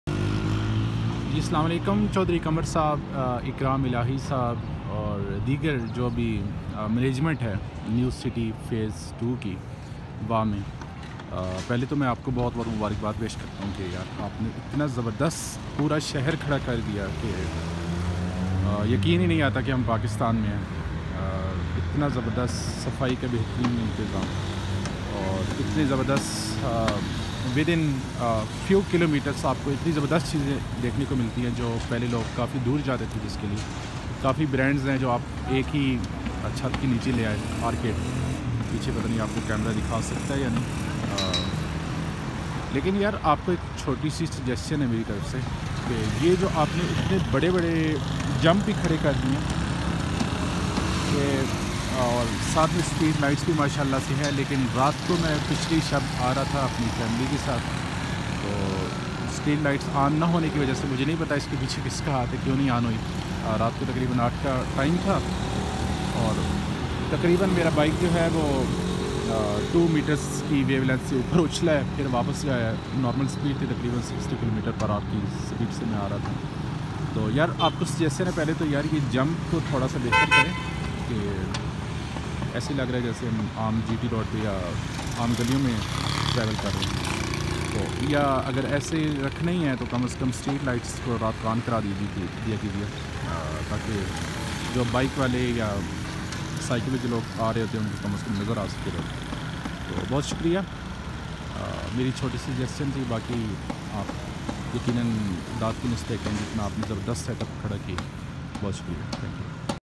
Suggestion to the Management team of New City Wah
Got a miner bike accident due to the poor management of New City Phase 2 | Killing Road Breakers